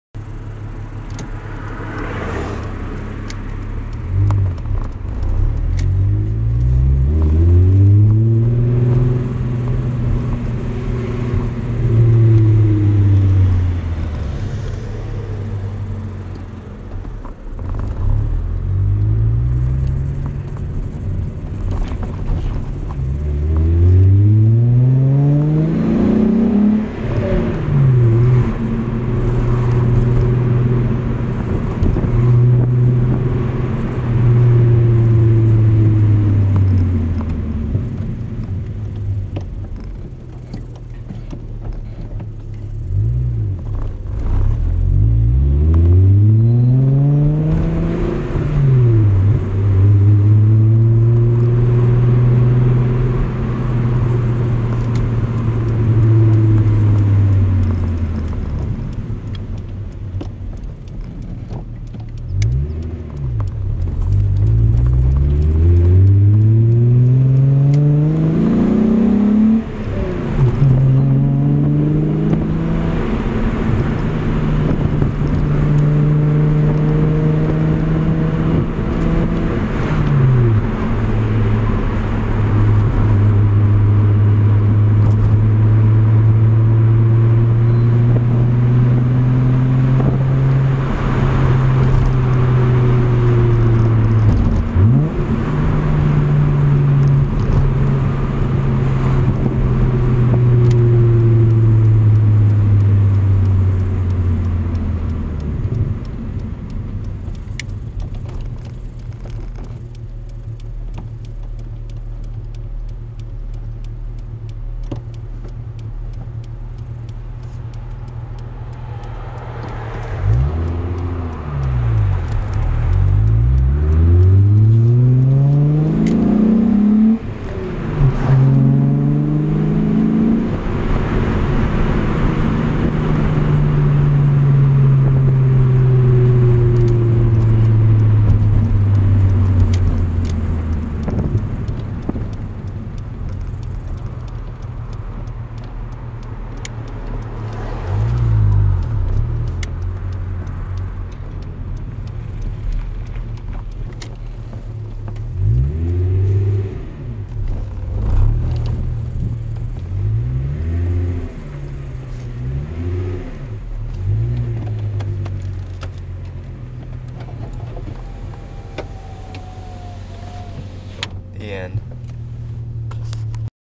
95' VW MK3 GTI VR6 (Drive Sound Test)
*TURN UP SOUND!* (Didn't have my license/permit during this video) I was still learning to drive stick.
VR6 sound test. My 95' Mk3 VR6. Had a custom intake and deleted res in this I believe.